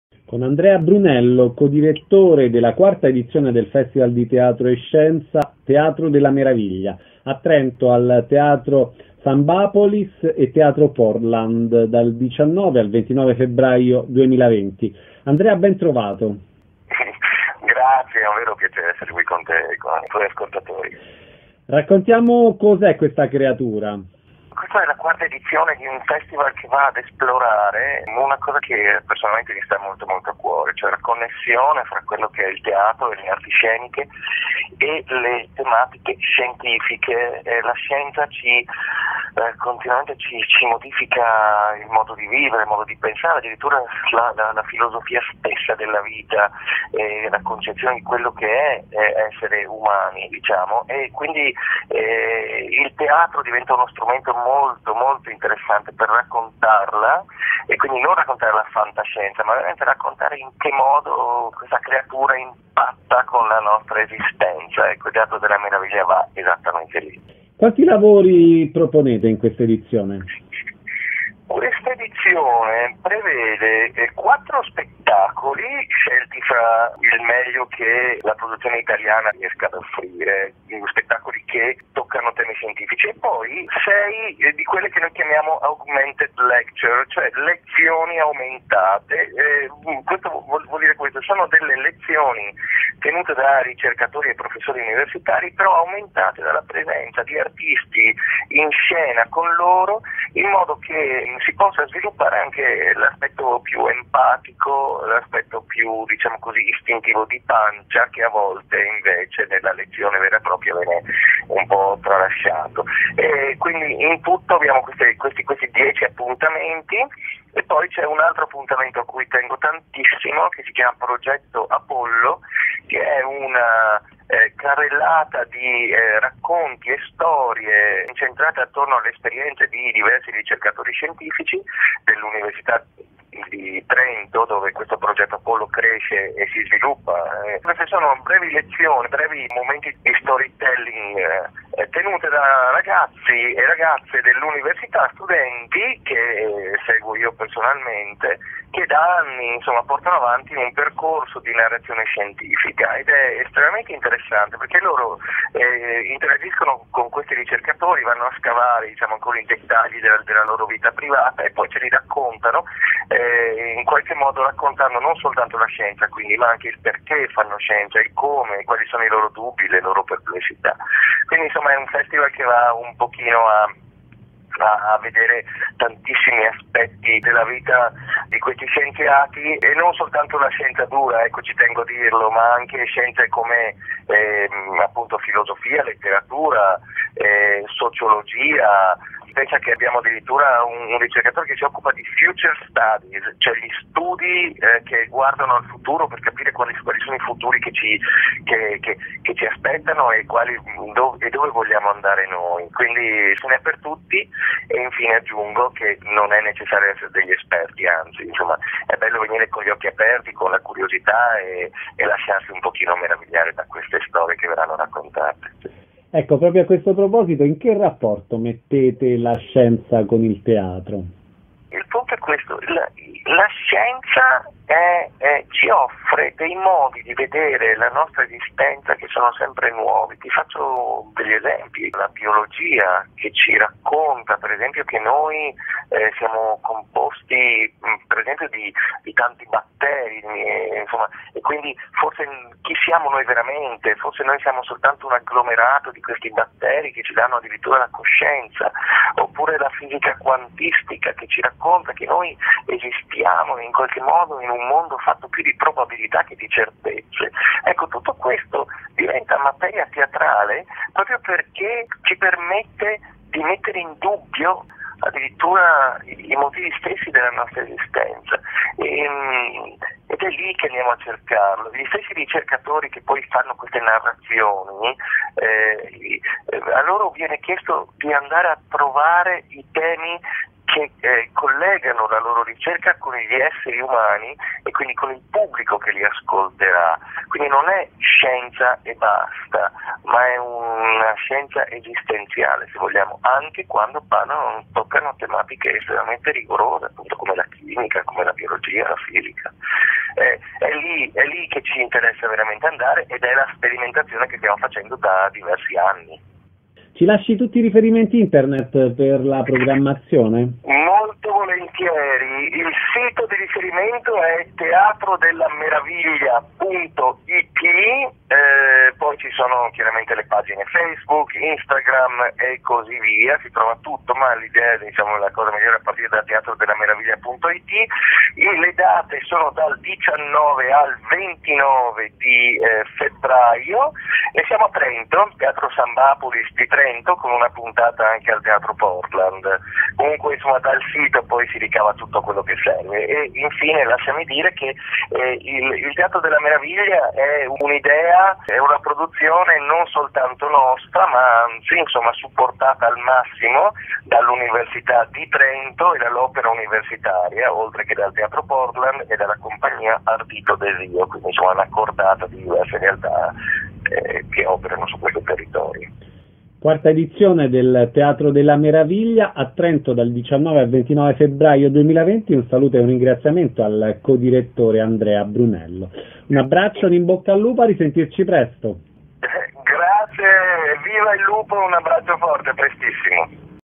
2020 02 15 - Andrea Brunello - Teatro della Meraviglia, 4ª edizione
Andrea Brunello, co-direttore, presenta la 4ª edizione del Festival di Teatro e Scienza
TEATRO DELLA MERAVIGLIA
(Trento - Teatro Sanbàpolis e Teatro Portland, 19-29 febbraio 2020)